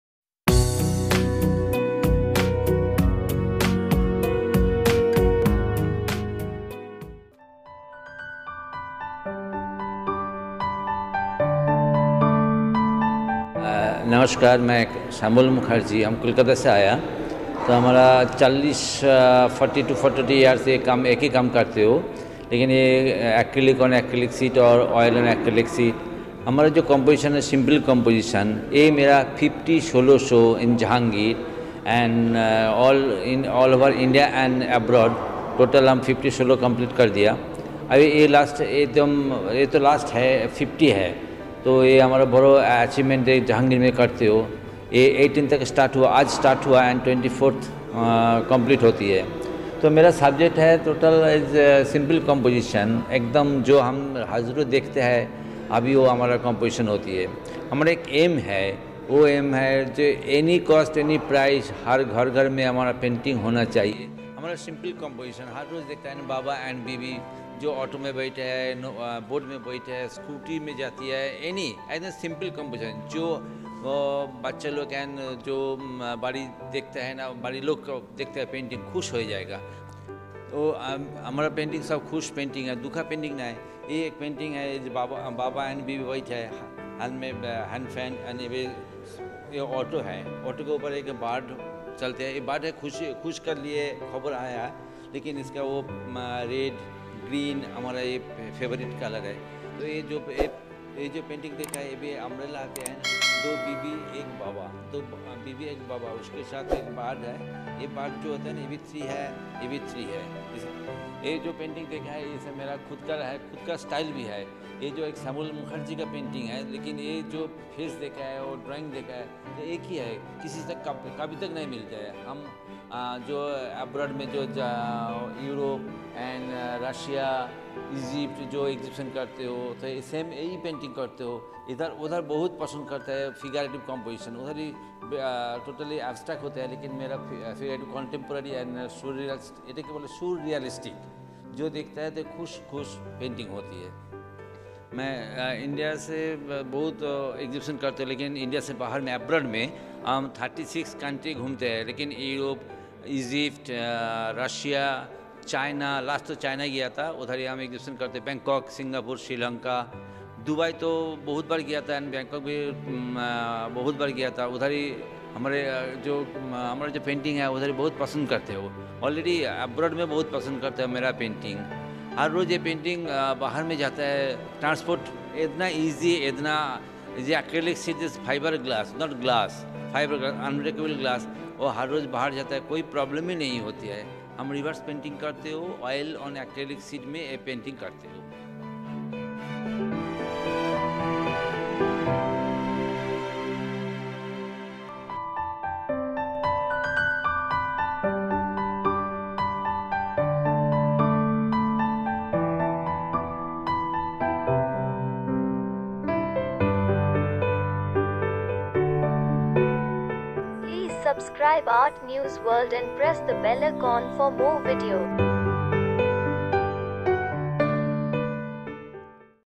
Baba & Bibi | Solo exhibition of Glass Paintings by Shyamal Mukherjee at Jehangir Art Gallery Mumbai
Artwork from artist Shyamal Mukherjee  Baba & Bibi series

On one hand, there is artist Shyamal Mukherjee, who has spent his entire life in Kolkata, only venturing out to attend showing off his works. the artist, who completed his Bachelor of Fine Arts (BFA) in 1987, and then his Master of Fine Arts (MFA) in 1989 from the Rabindra Bharati University, is known for his works on reverse oil or acrylic paintings on transparent acrylic sheets. expect to see a strong Bengal School figurative tradition being Carried ahead and interpreted in a contemporary way.

Most of my inspiration comes from observing people. In my baba & Bibi series of paintings, all you can see in happiness, there is not an iota of Grave in it. only happy and colourful figures.

He says. Most of the people Mukherjee's paints are performers of some type aur something all of us do in our day to day life, like going to the market or sitting at home.

His figures or dressed in some of the brightest shades of orange, face almost cartoon-like and their figures podgy, making a striking image. another interesting feature in his paintings are that his characters are in pairs or larger group, never alone.